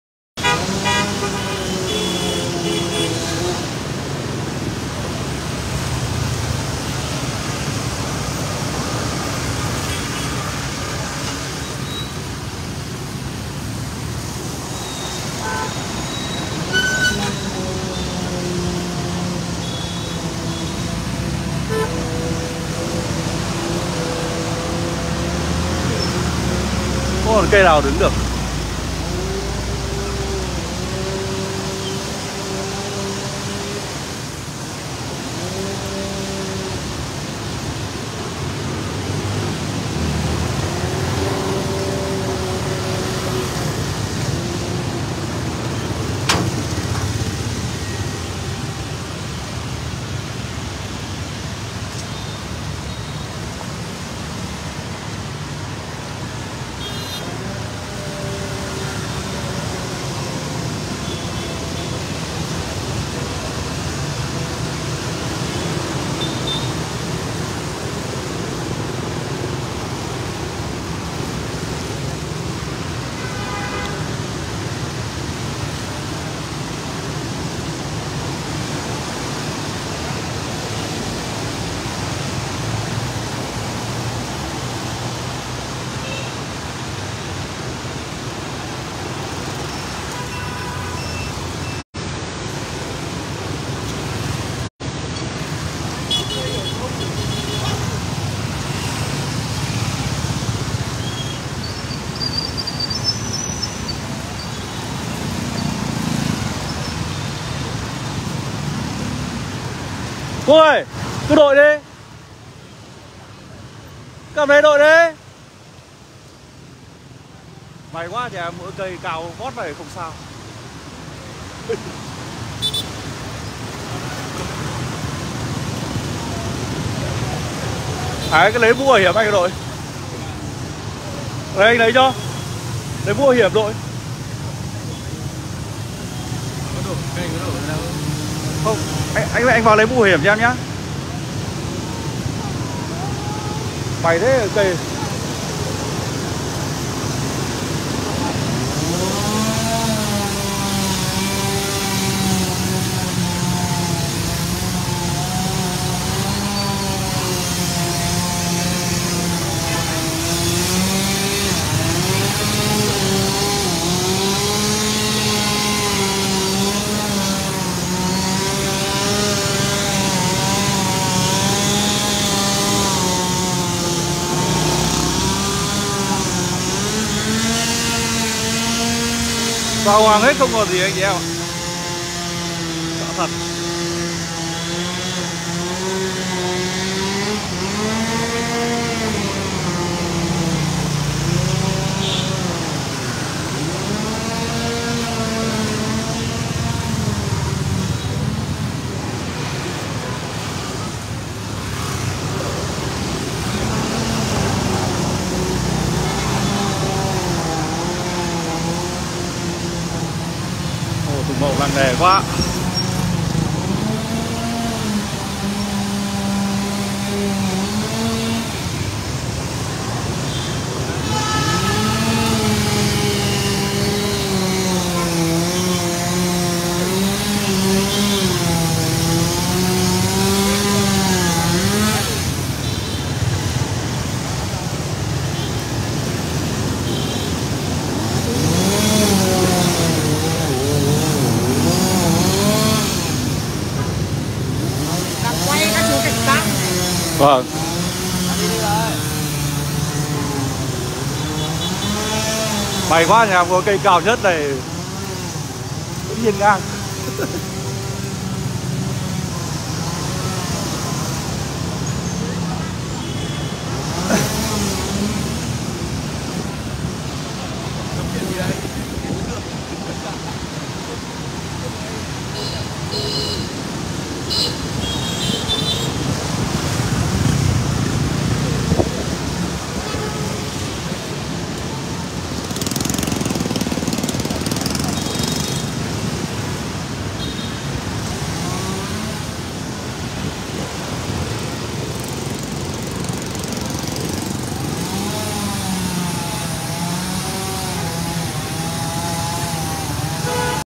Hồ Tùng Mậu Hà Nội tan hoang do cây đổ bởi bão Yagi bác Tô Lâm ạ, đặc biệt cây không được cắt tỉa
Hồ Tùng Mậu Hà Nội tan hoang do cậy đổ bởi bão Yagi bác Tô Lâm ạ, đặc biệt cây không được cắt tỉa

• 𝐒𝐮𝐧𝐬𝐞𝐭 𝐇𝐨𝐮𝐬𝐞 𝐏𝐡𝐮́ 𝐐𝐮𝐨̂́𝐜

• Address: 73 Positano - Sunset Town - Phú Quốc